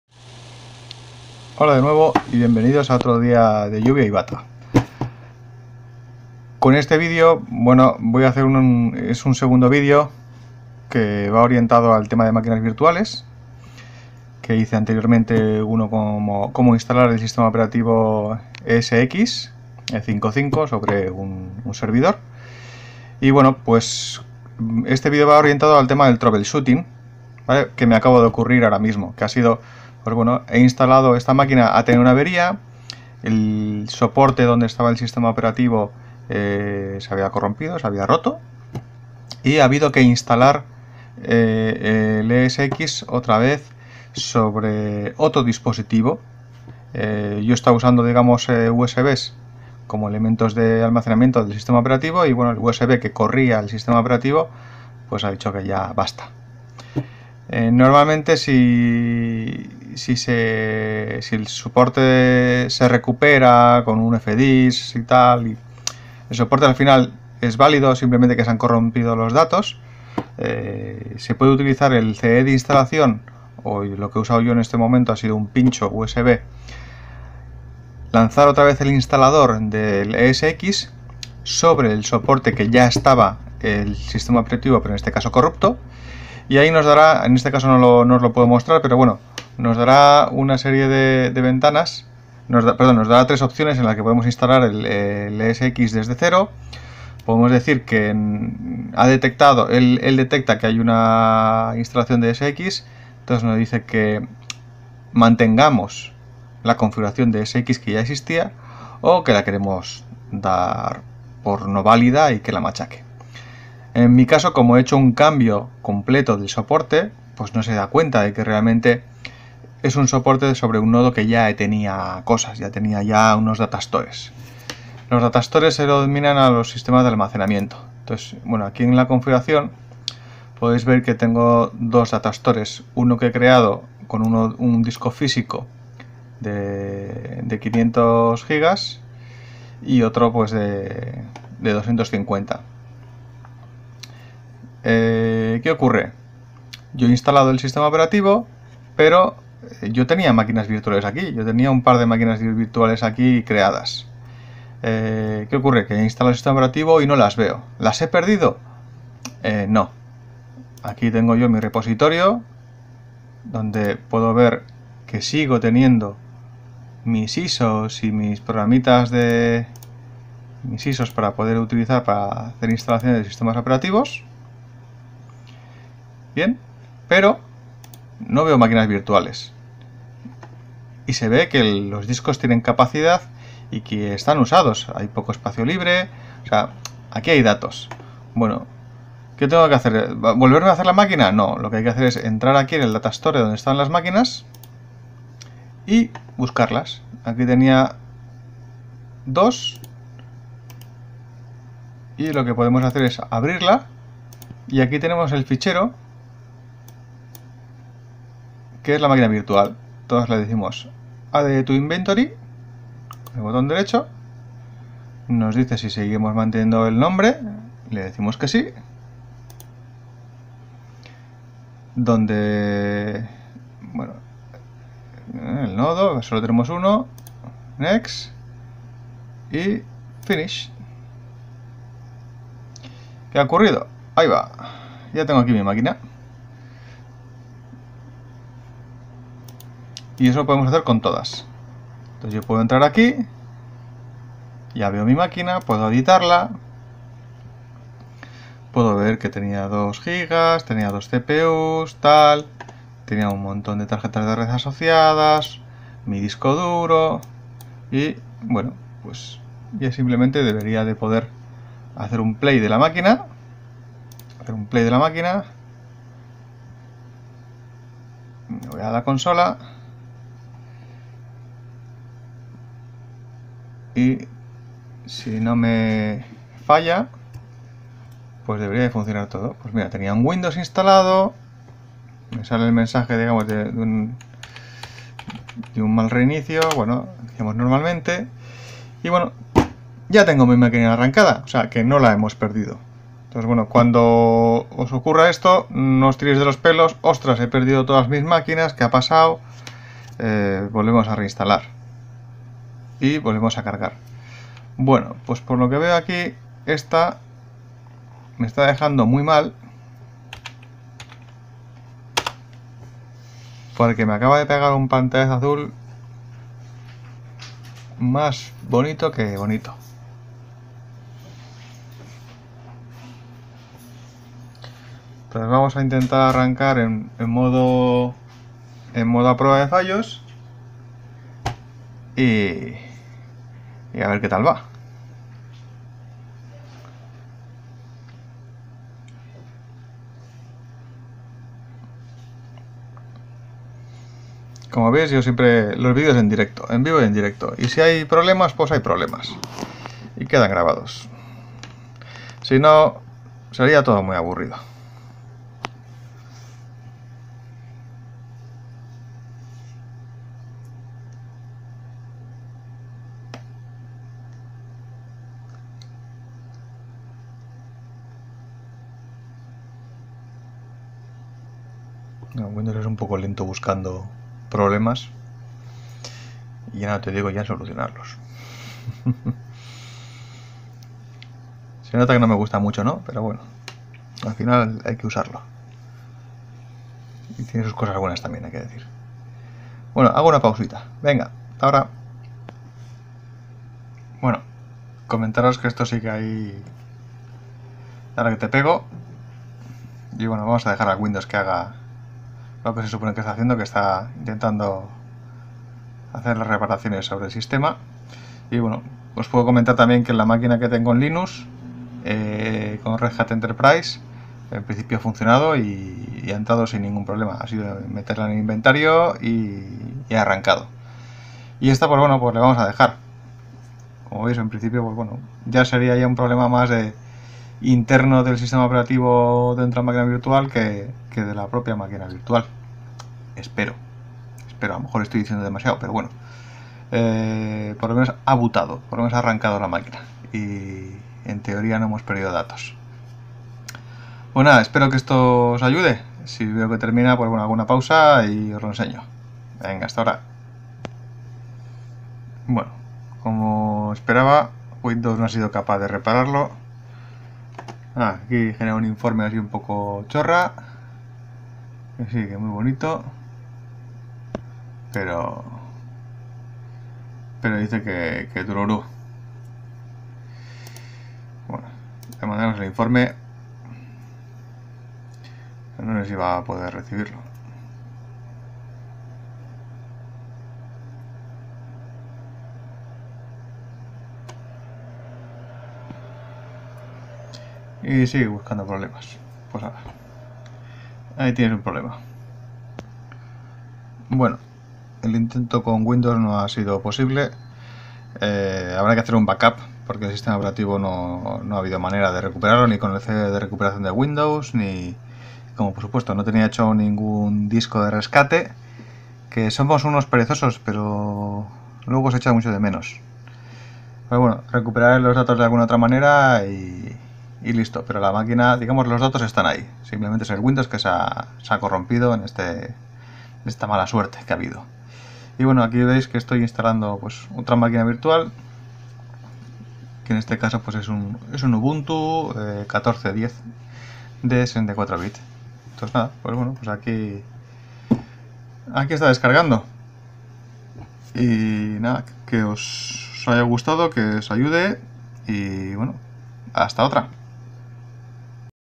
troubleshooting esxi 5.5: Perdida del S.O en un nodo
Como recuperar el control de tus máquinas virtuales y datastores tras la avería del HDD (en este caso USB) que corre el S.O ESXi de un nodo con Máquinas Virtuales VMware